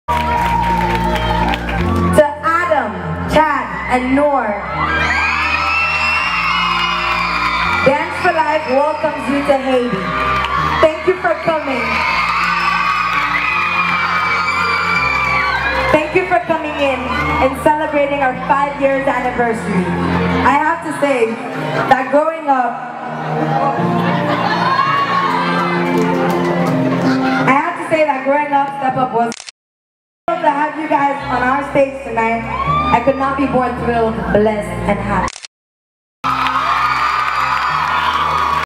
Haïti, Thank You!
"Every child deserves a champion – an adult who will never give up on them, who understands the power of connection and insists that they become the best that they can possibly be." Savina Saint Remy you have done that exact thing for all your dancers through the power of everything Dance For Life Studio represents and strives to achieve.
Thank you so much for your hospitality and the opportunity of taking part of your 5th anniversary TheNextGenerHaitian. Adam Sevani, Madd Chadd and I hope you all enjoyed the show and we look forward to seeing you and your wonderful team again soon!
It was such a pleasure collaborating with you and watching all the months of hard work come to such successful fruition, however the best part of working with you on your event, was leaving it with newfound friends.
A bientôt mes amis! x